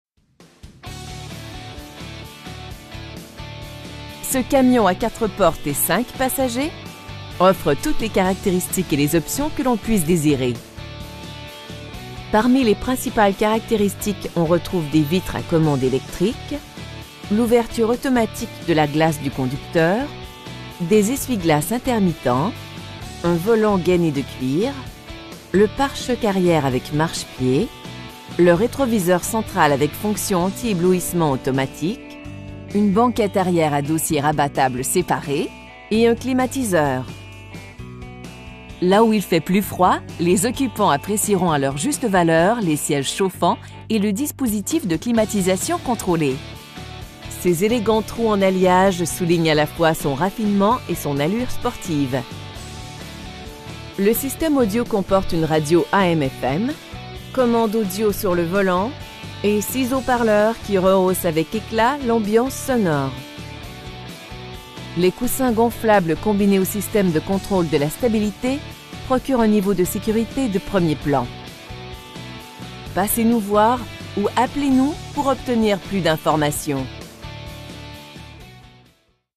2021 Ram 1500 Classic Night Sub-Zero in Laval, QC H7L2Z2
Ce camion à 4 portes et 5 passagers offre toutes les caractéristiques et les options que l'on puisse désirer. Parmi les principales caractéristiques on retrouve des vitres à commande électrique, l'ouverture automatique de la glace du conducteur, des essuie-glaces intermittents, un volant gainé de cuir, le pare-choc arrière avec marche-pied, le rétroviseur central avec fonction antiéblouissement automatique, une banquette arrière à dossiers rabattables séparés, et un climatiseur. Là où il fait plus froid, les occupants apprécieront à leur juste valeur les sièges chauffants et le dispositif de climatisation contrôlée. Ses élégantes roues en alliage soulignent à la fois son raffinement et son allure sportive. Le système audio comporte une radio AM/FM, commandes audio sur le volant, et 6 haut-parleurs qui rehaussent avec éclat l'ambiance sonore. Les rideaux gonflables combinés au système de contrôle de la stabilité procurent un niveau de sécurité de premier plan. Ce qui nous rend fiers, c'est vous offrir uniquement des véhicules de grande qualité. Passez nous voir ou appelez-nous pour obtenir plus d'information.